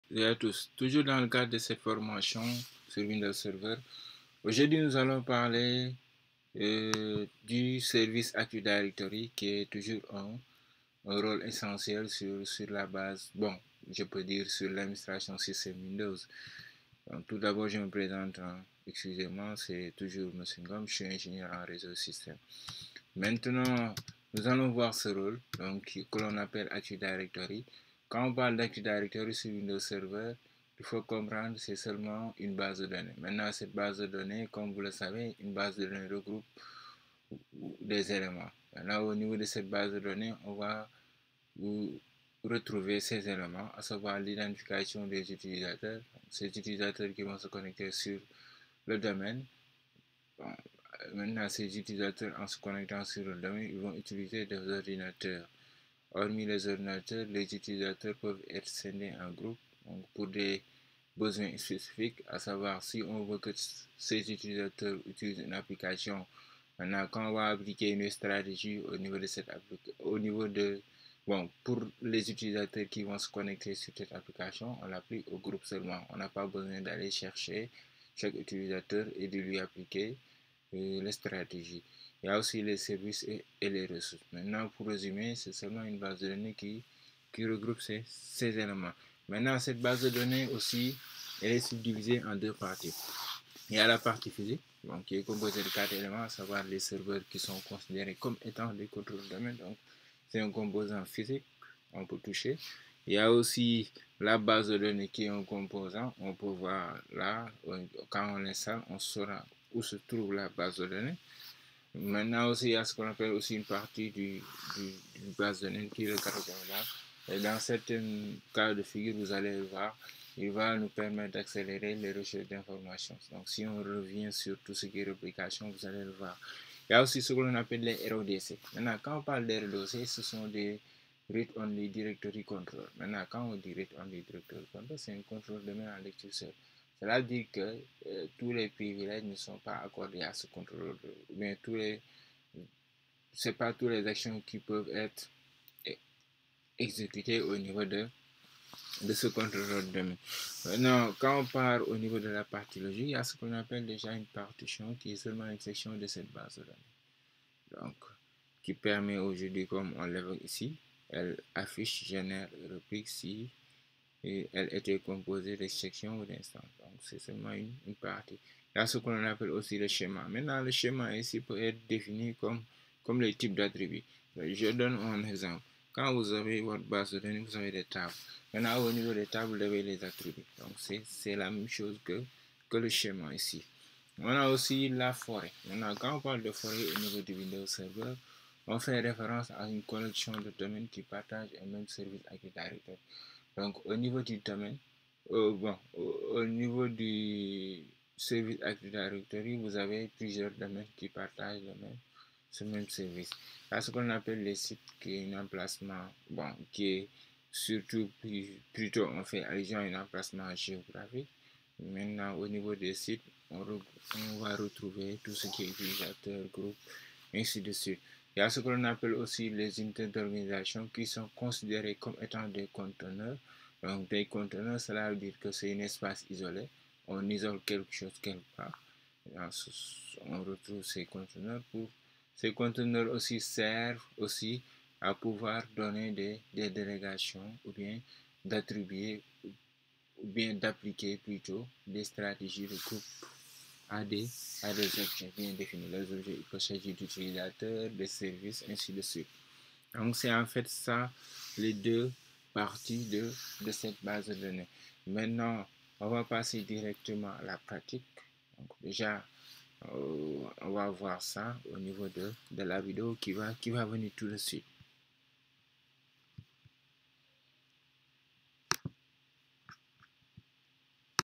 Presentation de Active Directory Windows server
Presentation du Role Active Directory ( AD DS ) et de ces composants .